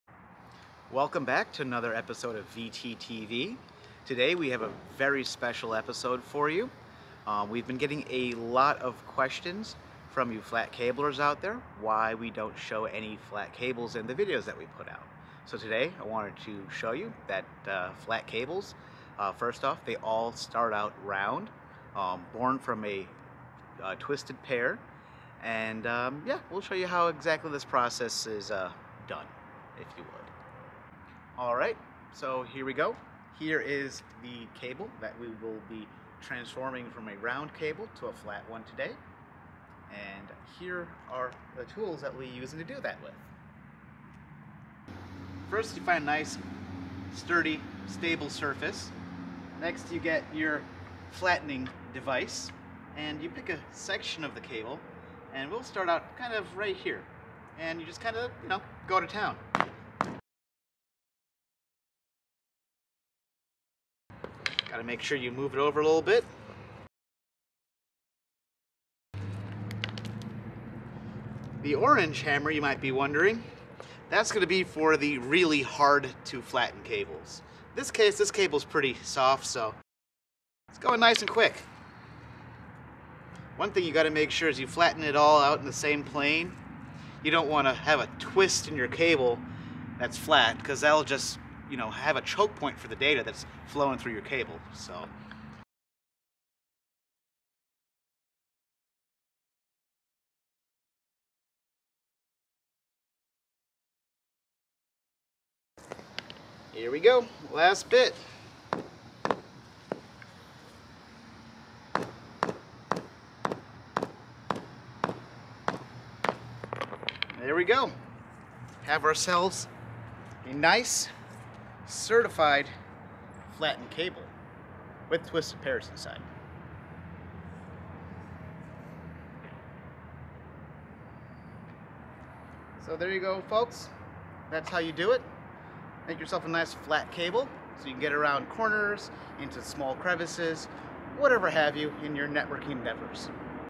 How to Flatten a Network Cable (Parody)
Well viewers, (some of) you asked for it, and we hope you enjoy our educational video on how to make flat ethernet cables!